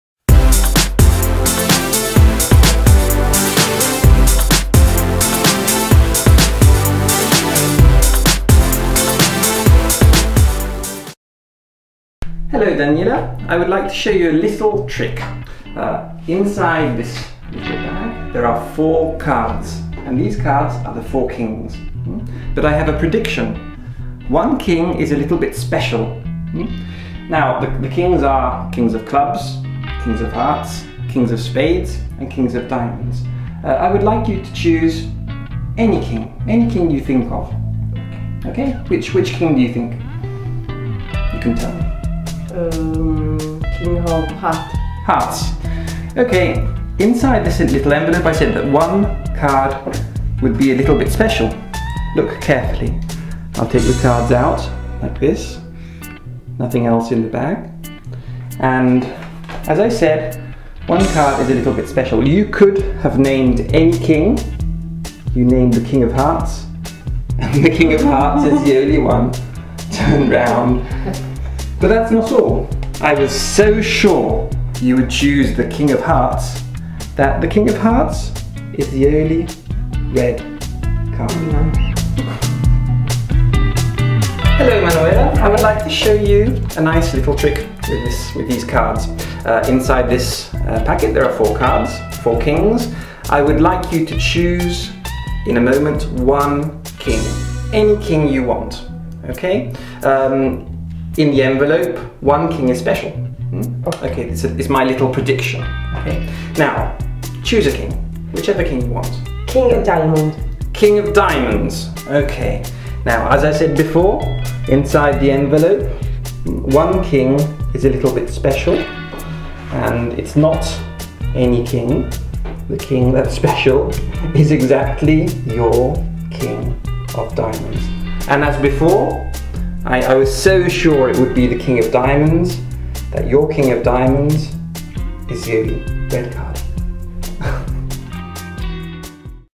Four kings (cuatro reyes)
Juego de cartas Four Kings, truco de magia muy fácil de realizar, produce una gran reacción ante el público, podrás realizarlo una y otra vez que siempre se sorprenderán...